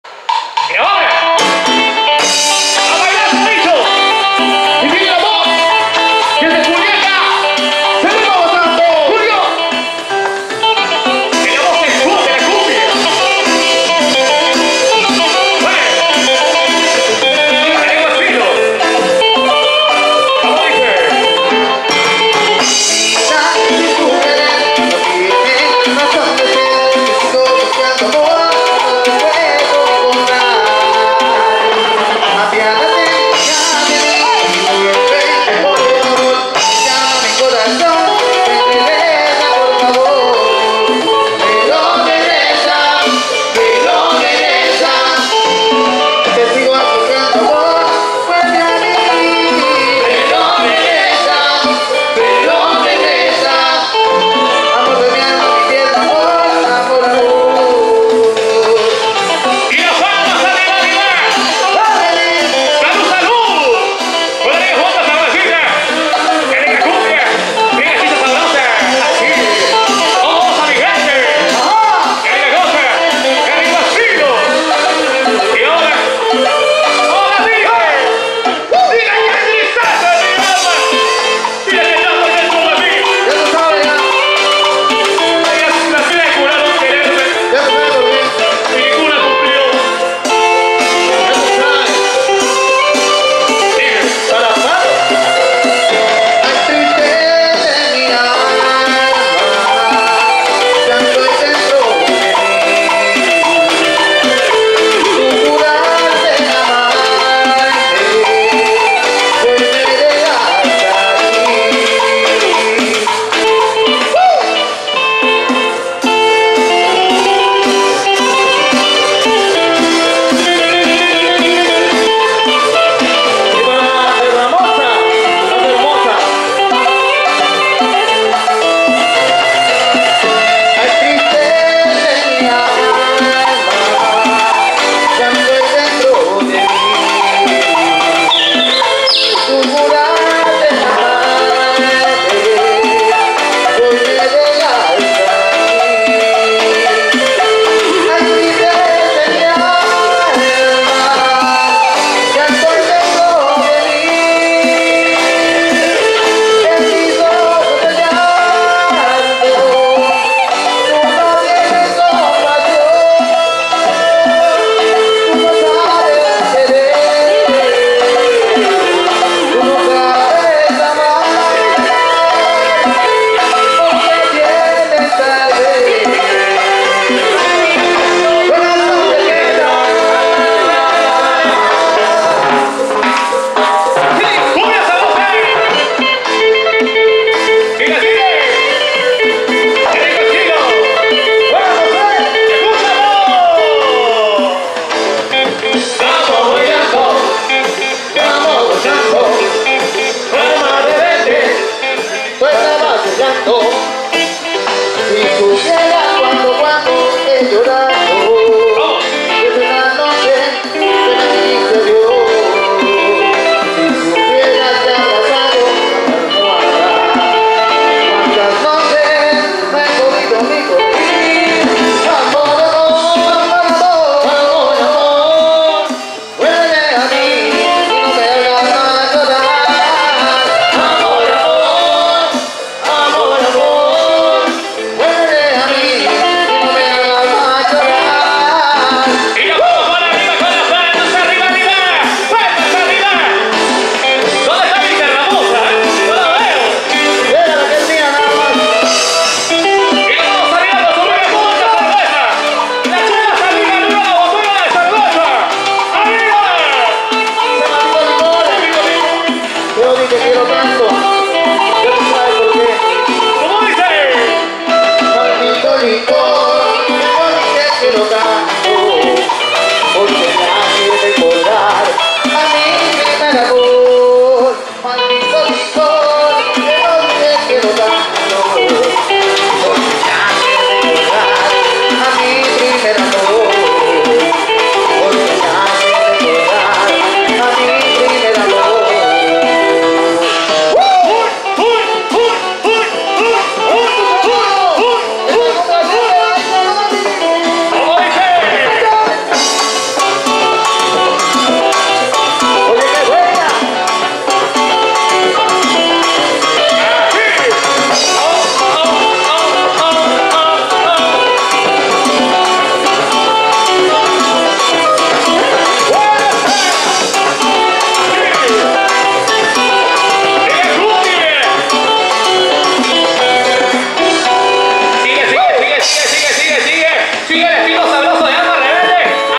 HD Aracely y su Alma Rebelde en Yarinacocha
ARACELY Y SUS BAILARINAS WENDY GREYSI Y KATHY PARA EL ANIVERSARIO DE LA COMUNA DE YARINACOCHA EN PUCALLPA.